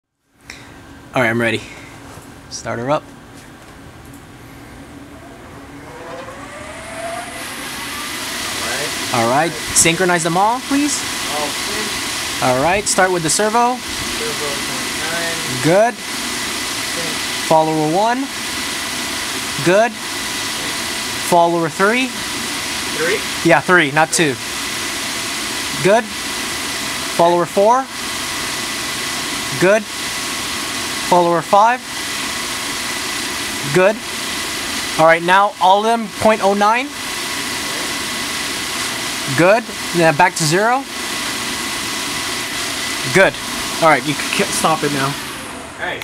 Sensorless Rotor Angle synchronization with 5 rotors - 20190620
Demonstration of rotor phase angle synchronization capability using LaunchPoint's motor controllers and sensorless COTs/hobby UAV motors and rotors